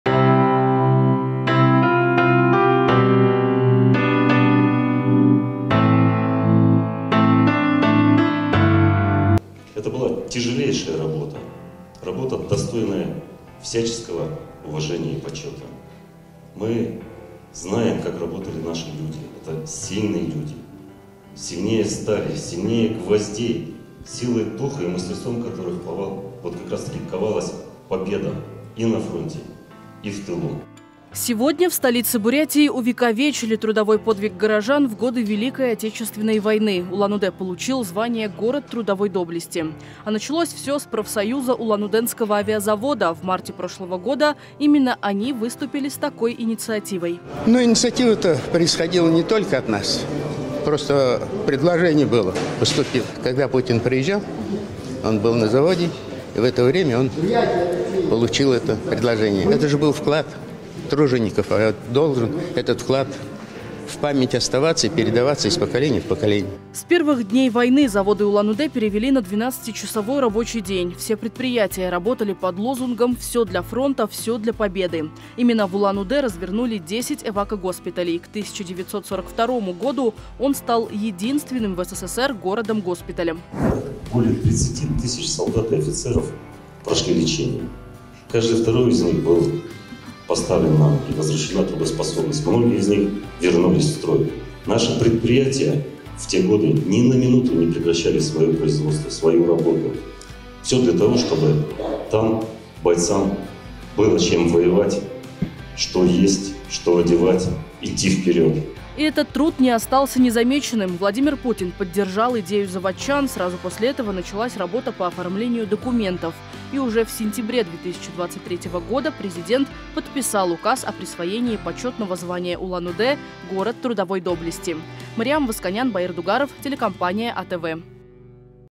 Город трудовой доблести. В Улан-Удэ открыли мемориальную доску
Улан-Удэ – город трудовой доблести. Теперь официально. Сегодня в администрации города открыли мемориальную доску в честь присвоения такого звания. Как это было, смотрите далее.